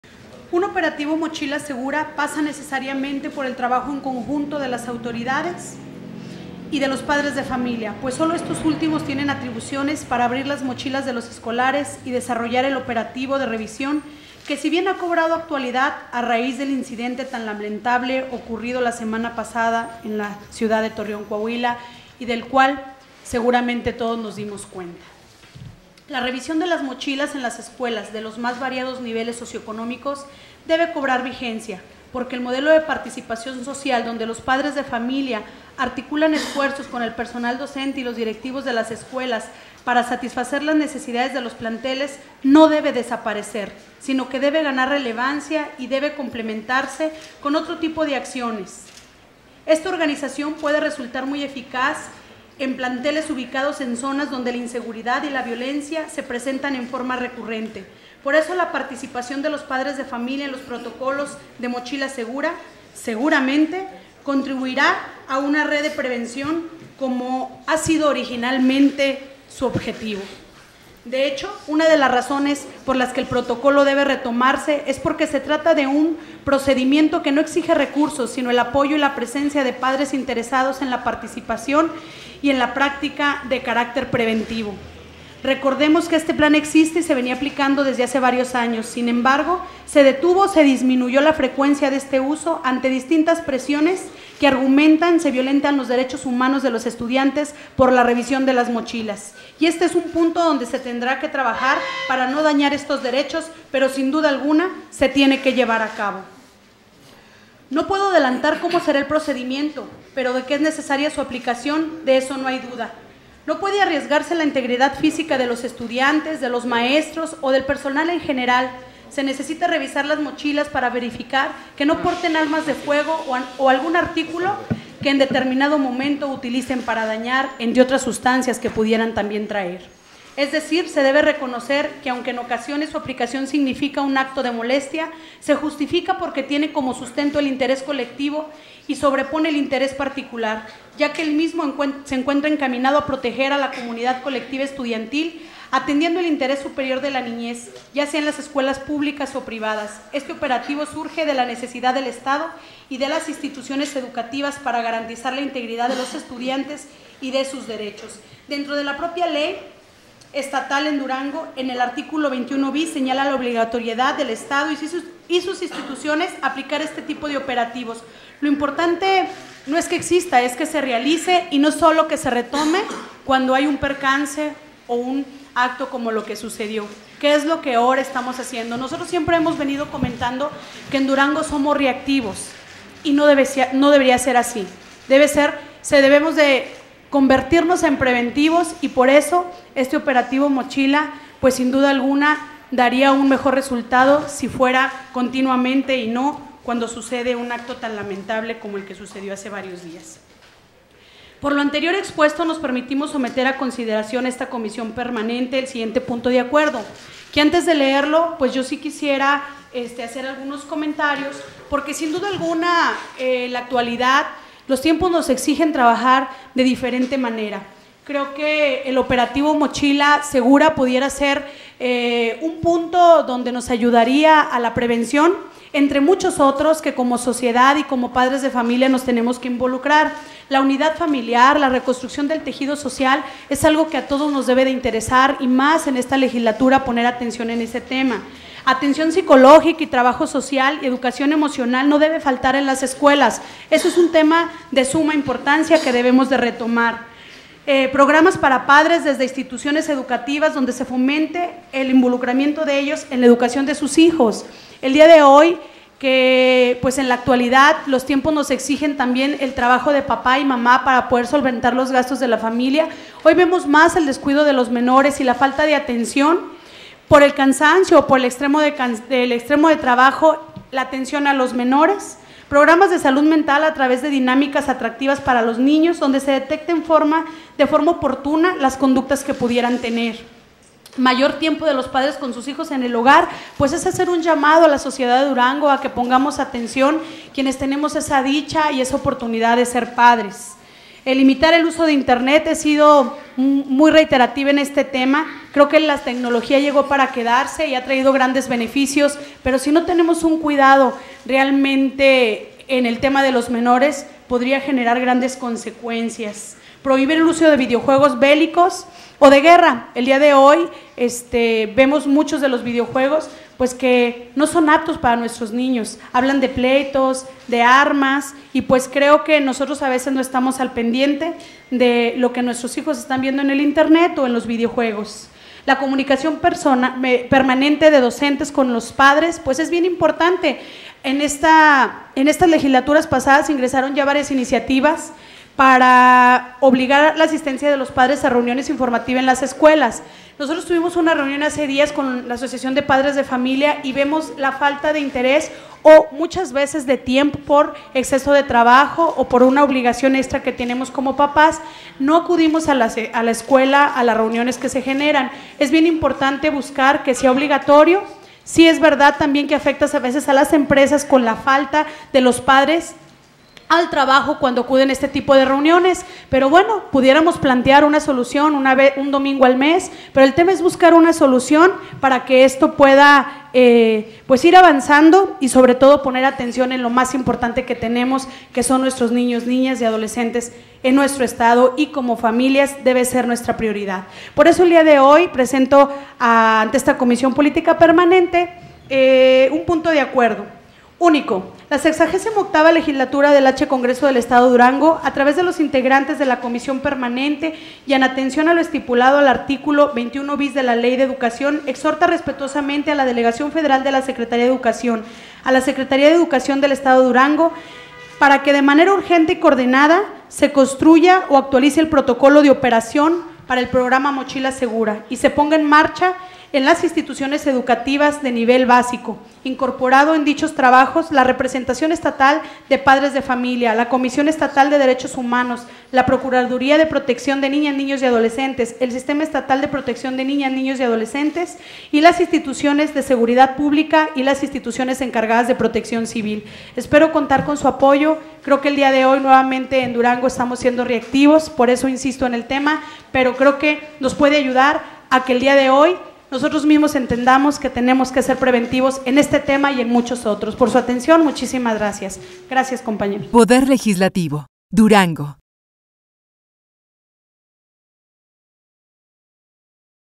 Dip. Alicia Guadalupe Gamboa Martínez, Comisión Permanente del 14 de enero de 2020, Punto de Acuerdo
Se presentó Punto de Acuerdo por la diputada Alicia Guadalupe Gamboa Martínez y los integrantes del GPdelPRI, en el que emitió un exhorto a las autoridades educativas, para que de manera urgente, se aplique el protocolo de operación “Mochila Segura”